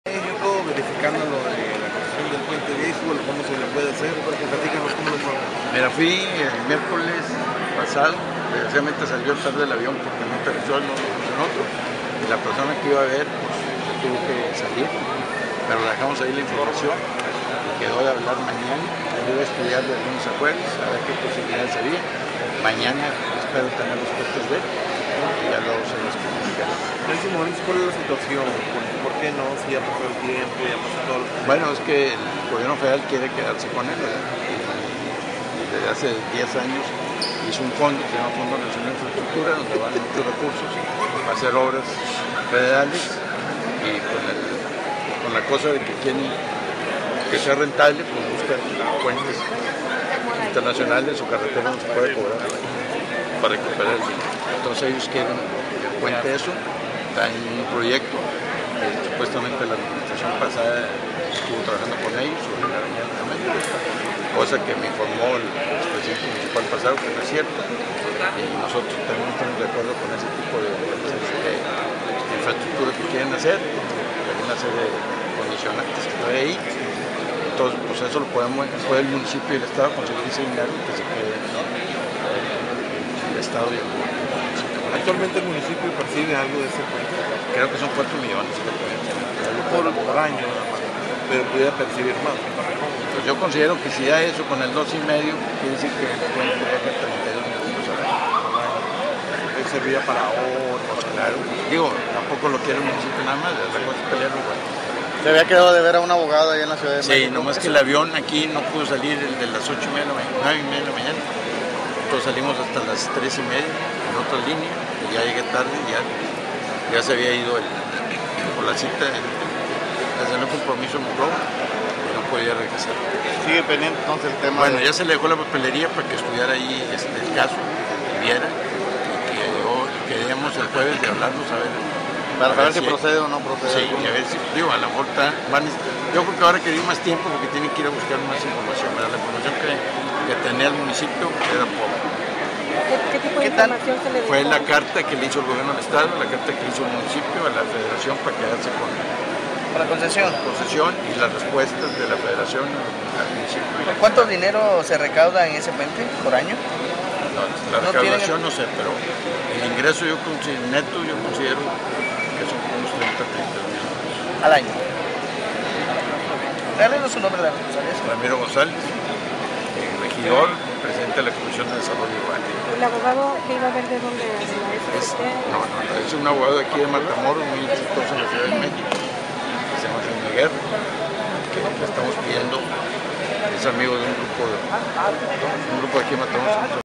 Sin respuesta de la concesión del Puente Viejo
Con el tema de la concesión del puente Internacional Matamoros / Brownsville, es que se demandará jurídicamente al gobierno federal, en este caso a la Secretaría de Comunicaciones y Transportes.
Hasta ahora la postura de esa dependencia federal es de concesionar el puerto fronterizo al Fondo Nacional de Infraestructura (FONADIN).
En entrevista Ramiro González Garza, primer regidor en el ayuntamiento y presidente de la Comisión de desarrollo urbano, para la concesión del Puente Viejo Internacional, manifestó que se peleará jurídicamente la adquisición de ese puerto fronterizo, porque el municipio de Matamoros tiene derecho.
Se le cuestionó al Regidor el por qué, de no obtener respuesta después de ya tiempo por parte del gobierno Federal, a lo que respondió:
"El gobierno Federal, quiere quedarse con él, desde  hace 10 años se hizo un fondo, se llama Fondo Nacional de Infraestructura; que es a donde van muchos recursos para hacer obras federales, y con el conque, que quieren que sea rentable, pues buscan puentes internacionales o carreteras donde se pueda cobrar para recuperar, entonces ellos quieren eso del puente, traen un proyecto que supuestamente la administración pasada se estuvo trabajando con ellos, cosa que me platico el presidente de la administración pasada que no es cierto".
En un aproximado el municipio recibe alrededor de cuatro millones, anuales del puente.